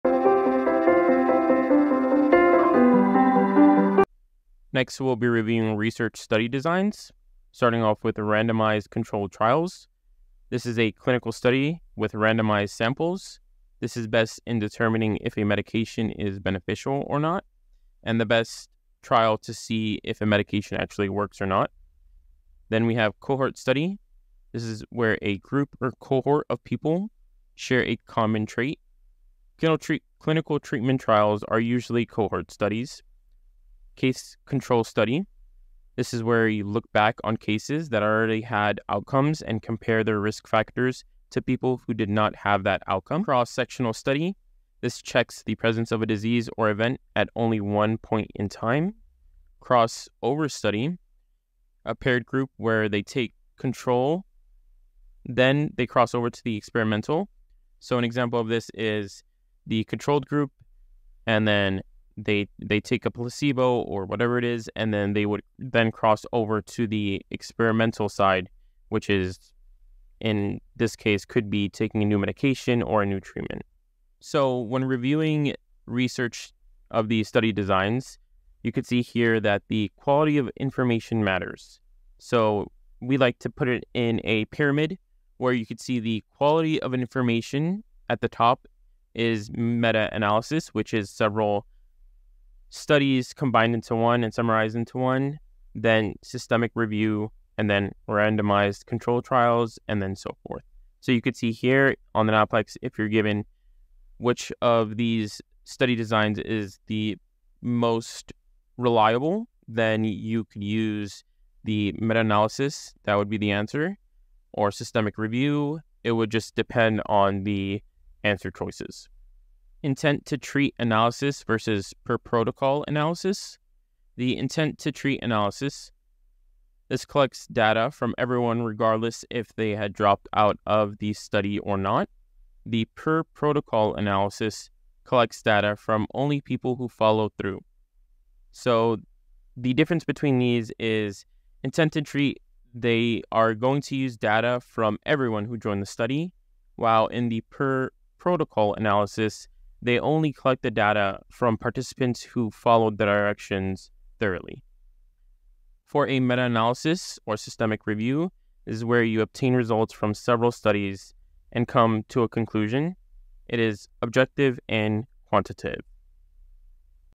Mastering Biostatistics Study Designs: A Pharmacist's Guide to NAPLEX Success
Welcome to our comprehensive guide on Biostatistics Study Designs, tailored specifically for pharmacy students preparing for the NAPLEX exam. In this intelligently crafted video, we delve deep into the intricacies of study designs, equipping you with the knowledge and expertise needed to ace the NAPLEX.

📚 What You'll Learn:

Understanding the Fundamentals of Study Designs
Types of Observational Studies: Cohort, Case-Control, and Cross-Sectional
Experimental Study Designs: Randomized Controlled Trials (RCTs)
Critical Appraisal of Research: Evaluating Study Quality
Practical Examples and Case Studies
Whether you're a seasoned pharmacy student or just beginning your NAPLEX journey, our expert insights and tips will elevate your understanding of biostatistics study designs, ensuring you're well-prepared for the exam.

Don't miss this opportunity to boost your NAPLEX readiness and excel in your pharmacy career. 📊💊🧪